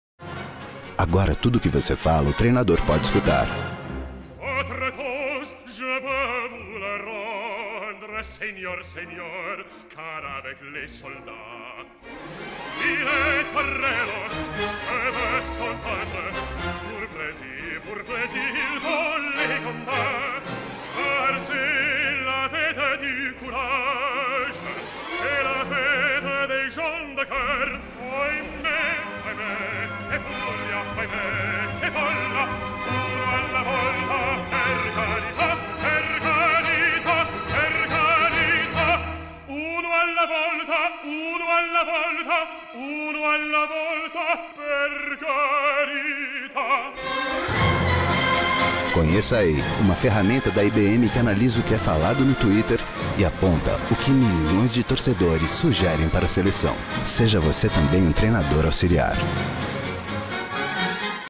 IBM - Ei! - TV Spot
IBM Ei! Confederation Cup Campaign

At the Confederation Cup in Brazil we created an app for one person only. Ei! is an app that identifies, filters and analyzes all the social comments that Brazilians have made about the team, players and games, delivering  advice on the most important decisions.With the touch of a button, Ei! knows what the country wants the couch to do, turning 200 million Brazilian fans into assistant coaches.

* If only the Brazilian soccer team coach had used Ei! during the World Cup too...